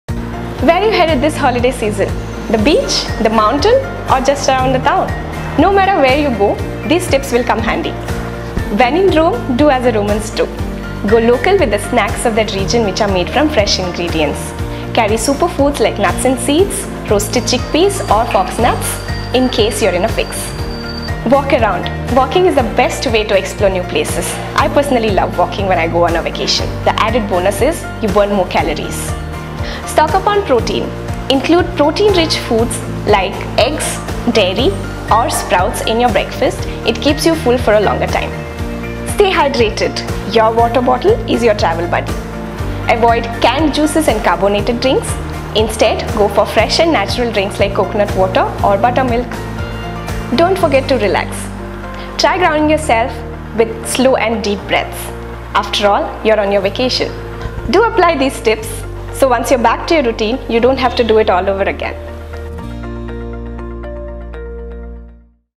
Tips To Maintain Diet While On A Vacation | HealthifyMe
Worried about going off track with your diet on a vacation? These diet tips by our Senior Nutritionist - Dr. Priyanka Marakini will come handy while on a vacation!

Available on Android and iOS.